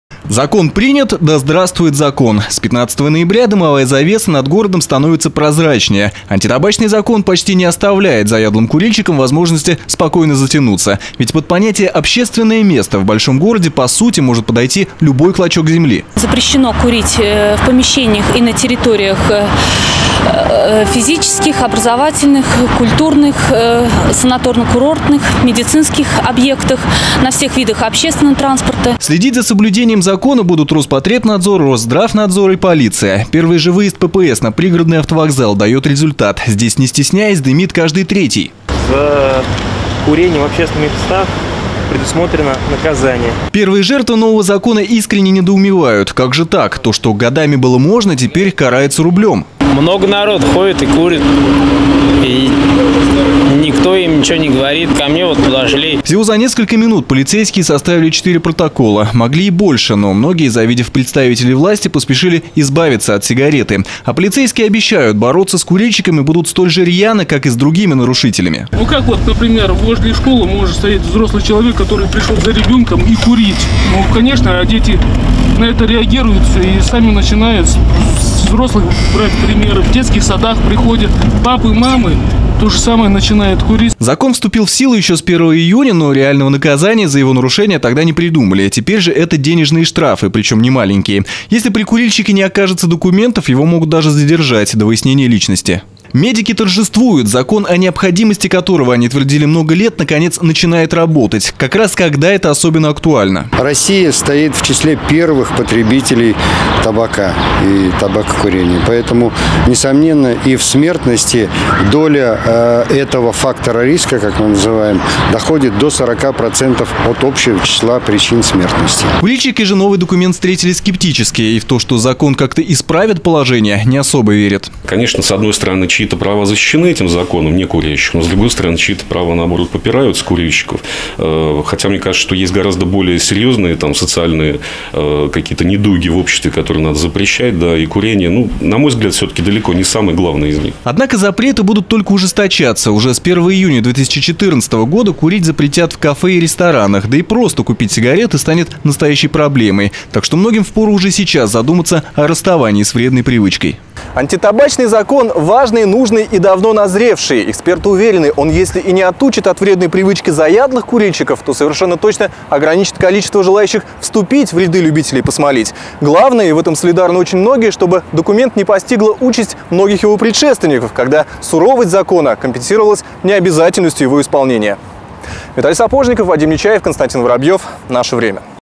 17 11 13 Табачное табу
Тяжелые времена для поклонников легкого кайфа. На неделе любителей покурить в общественных местах начали наказывать рублем. Отныне смолить запрещено на вокзалах, в учреждениях образования, культуры и спорта, в лифтах и подъездах домов, а также в пассажирском транспорте. За нарушение закона штраф — от пятисот до трех тысяч рублей. Ограничения с восторгом восприняли сторонники здорового образа жизни. А вот курильщики, их в Оренбурге немало, уверены: запрет на табак нарушает их права.